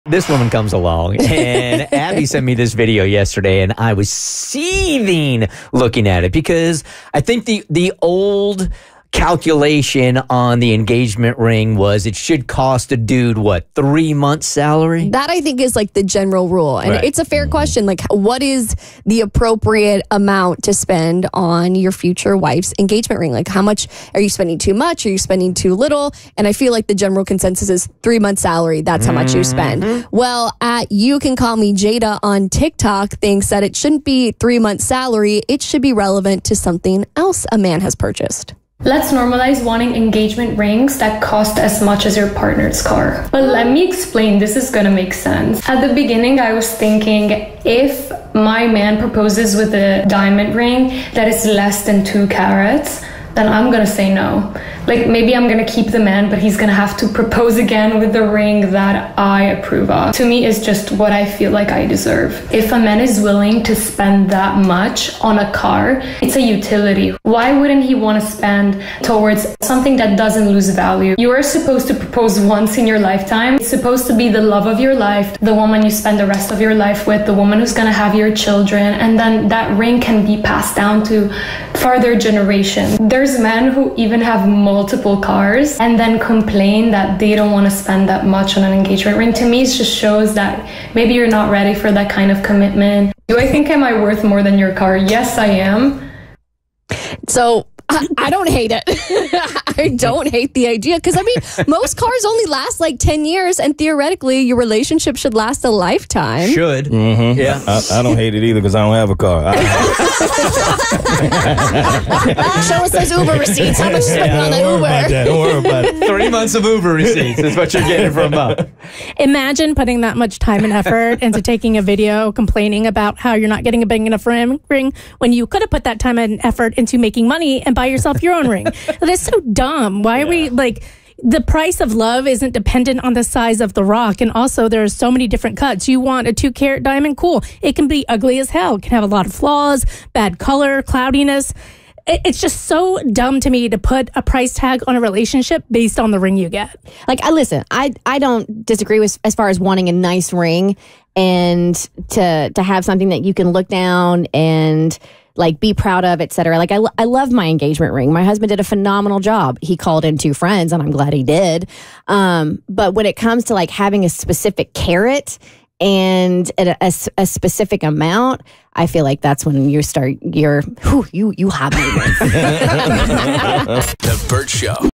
Do You Agree With This New Engagement Ring Rule?
The “Car Rule” for engagement ring buying claims that a man should spend as much as their car cost on an engagement ring! Do you agree?